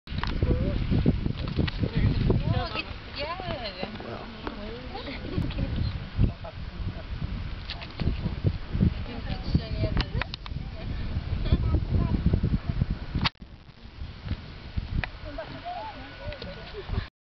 Turkmen fertility ritual
A Turkmen fertility ritual performed in Konye Urgench